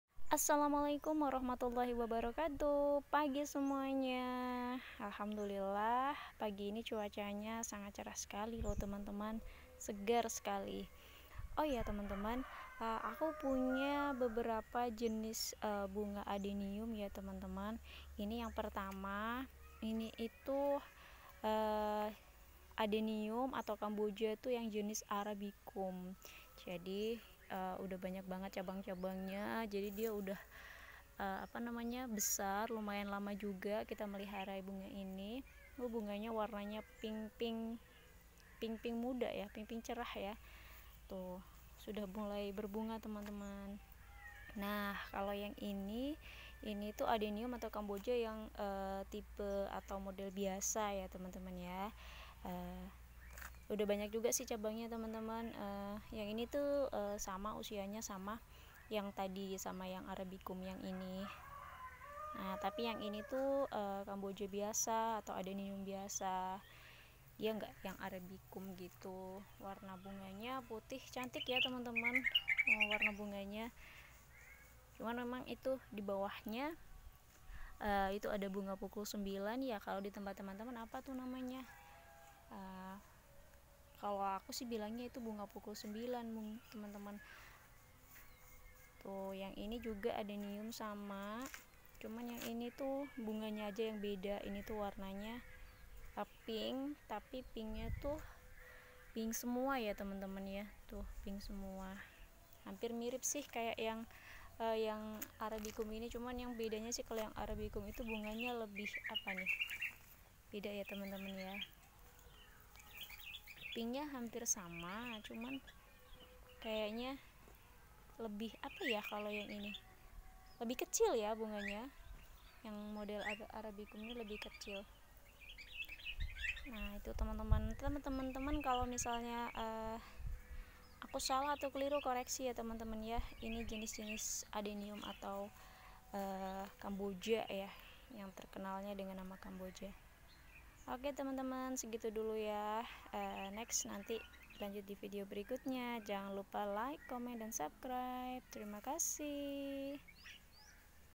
Kamboja ku oh adeniumku🌺Bunga Kamboja arabicum dan Adenium bonsai🌳
Dirumahku ada banyak macam2 bunga teman2..ada sansievera,sakura,pukul sembilan,keladi2an,Kamboja dan lain2..

Nah divideo kali ini aku mau ngasih tau tentang jenis bunga adeniun atau kamboja.
Untuk temen2 suka bunga yuk cus liat videonya ya..

Selamat menonton..
Semoga terhibur